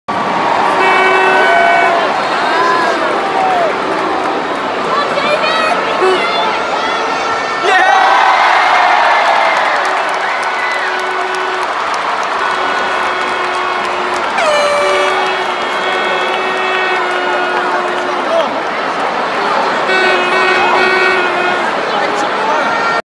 Hayden Christensen Penalty Socceraid 2018
After 90 minutes England & The Rest of the World were tied at 3-3 so it went to penalties